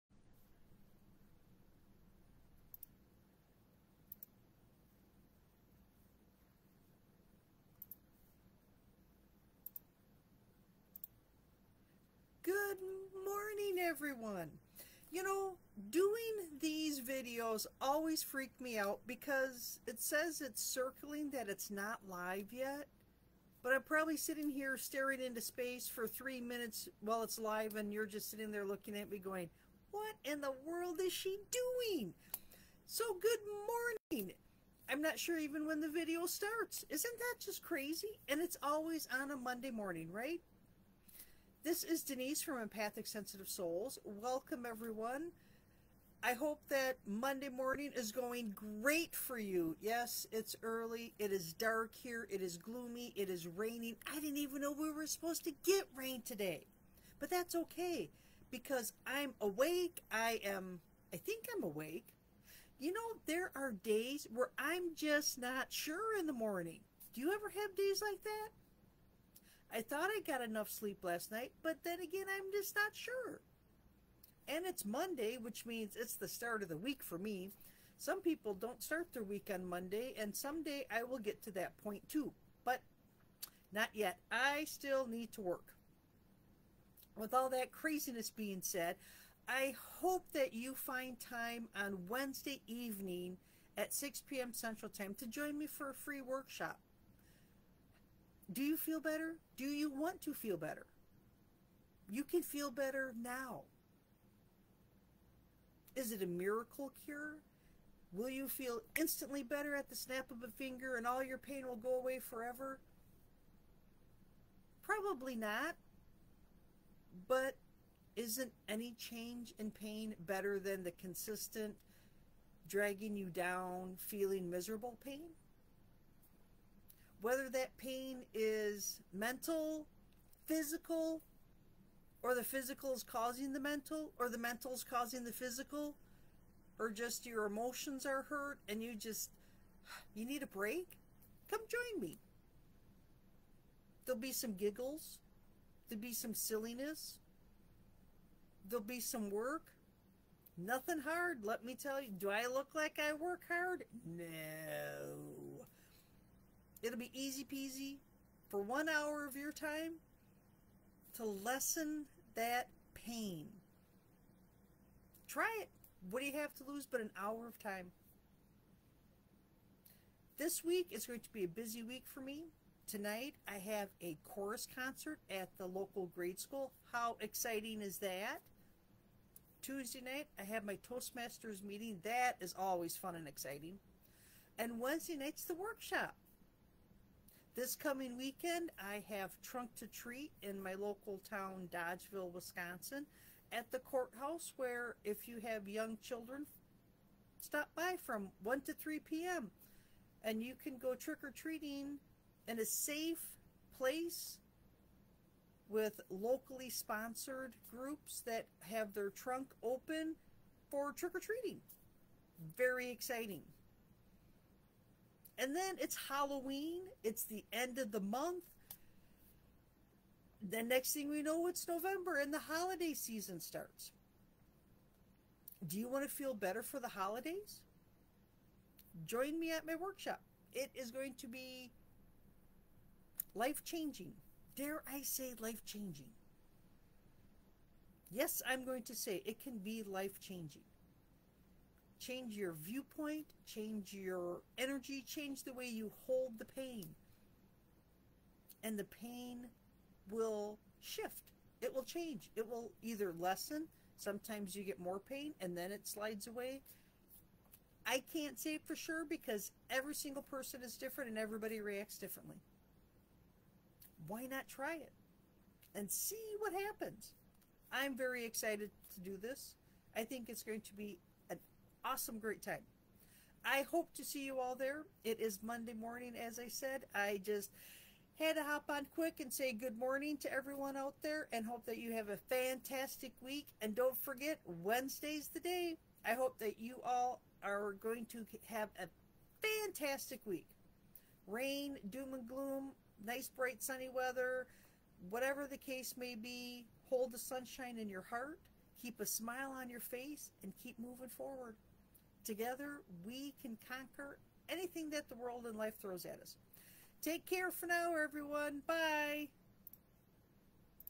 Unlock the Power to Feel Better Now: Join the Workshop!
Join me this Wednesday, 10/25/23 at 6 pm CT, for a FREE workshop on transforming your well-being! 🌟 Whether it's emotional, physical, mental, or spiritual pain, I'll show you that you CAN feel better. Don't miss this chance to make a positive change in your life. See you there!

Would you like a copy of my free Morning Mantras

Would you like to learn more about Denise?